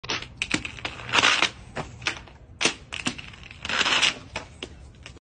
Use a car tire for a hockey rebounder?
Hey what ever it takes... Just don't dent the car!   Grind it out and be the best part of you.   Do what ever it takes.   Yes, this is totally ghetto... but is it?   Be resourceful and stay strong.     Nothing is handed to you.